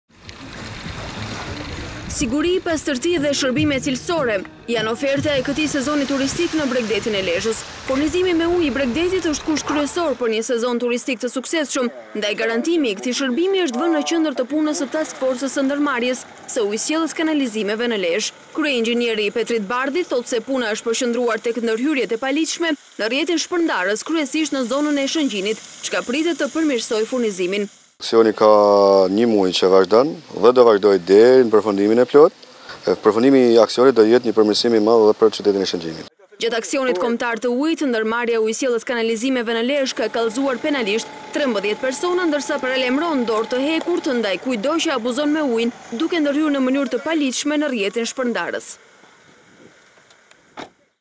Ora News - Ujë për sezonin, task-forca e Lezhës aksion kundër abuzimit në Shëngjin
Furnizimi me ujë është një prej shërbimeve kryesore nga të cilat varet suksesi apo dështimi i sezonit turistik për zonat bregdetare të vendit. Ndaj Ujësjellësi i Lezhës këto muaj të nxehtë ka vënë në fokus të punës përmirësimin në Shëngjin e Tale, dy prej plazhve më të populluara të Bashkisë. Në kuadër të aksionit kundër lidhjeve të paligjshme, 13 subjekte janë kallzuar për abuzime me ujin.

Siguri, pastërti dhe shërbime cilësore janë oferta e këtij sezoni turistik në bregdetin e Lezhës.

Furnizimi me ujë i bregdetit është kusht kryesor për një sezon turistik të suksseshëm, ndaj garantimi i këtij shërbimi është vënë në qendër të punës së Taks-Forcës së Ndërmarrjes së Ujësjellës Kanalizimeve në Lezhë.

Kryeinxhinieri i Petrit Bardhi thotë se puna është përqëndruar tek ndërhyrjet e paligjshme në rrjetin shpërdarës kryesisht në zonën e Shëngjinit, çka pritet të përmirësojë furnizimin.

Petrit Bardhi: Aksioni ka një muaj që vazhdon dhe do të vazhdojë derin në përfundimin e plotë. Përfundimi i aksionit do të sjellë një përmirësim të madh edhe për qytetin e Shëngjinit.

Gjatë aksionit Kombëtar të Ujit në Ndërmarja e Ujësjellës Kanalizimeve në Lezhë ka kallzuar penalisht 13 persona, ndërsa paralajmëron se dora e hekurt ndaj kujdo që abuzon me ujin, duke ndërhyrë në mënyrë të paligjshme në rrjetin shpërndarës.